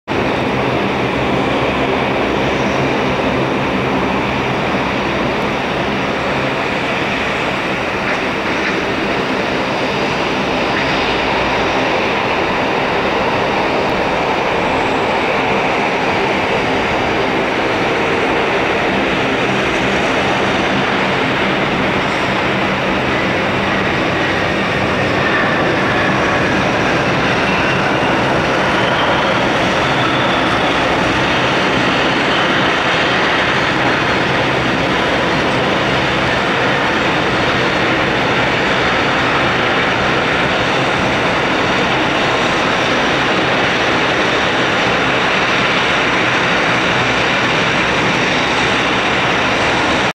Buddy On Board: American Eagle (Compass Airlines) ERJ-175 [N210NN] Landing Portland Airport (PDX)
Flight: CPZ6026 LAX-PDX
July 17, 2016
Landing on runway 28L with Planesguy on board.